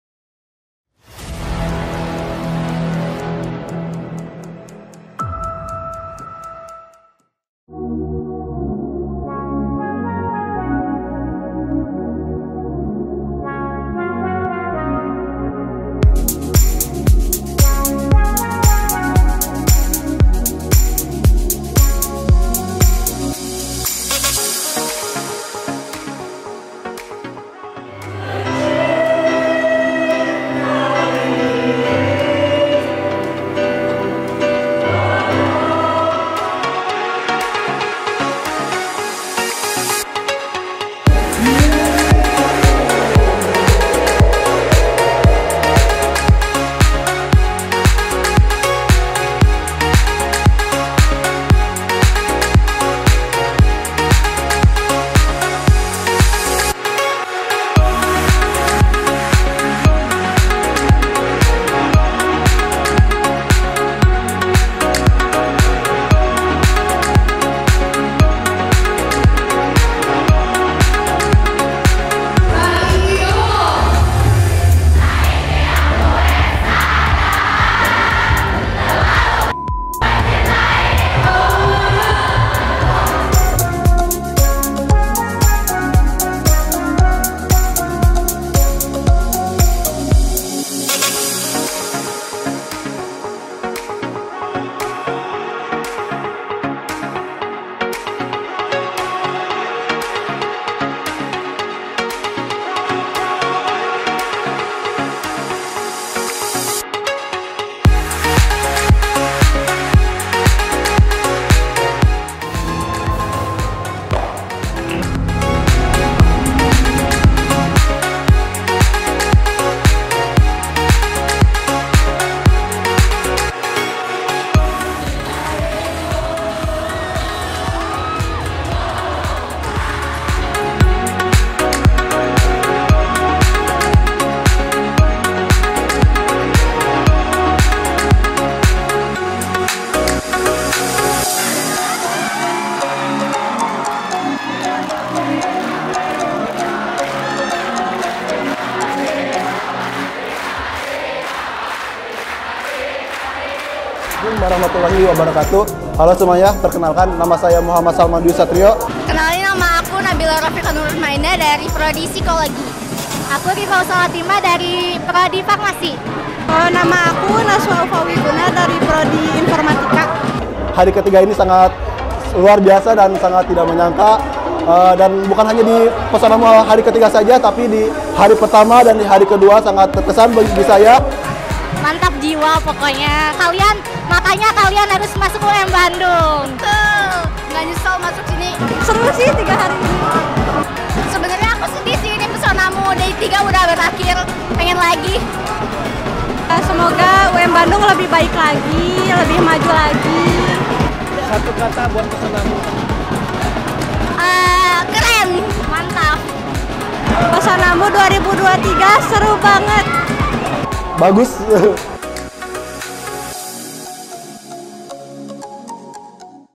Highlight DAY 3 PESONAMU 2023
Halo Bestie UMBandung, ga kerasa udh kuliah lagi ya. Sambil nunggu kelas, bestie UMBandung bisa nih nonton Highlight PESONAMU hari ketiga nih.

@Universitas Muhammadiyah Bandung

Untuk Informasi Penerimaan Mahasiswa Baru, Hubungi :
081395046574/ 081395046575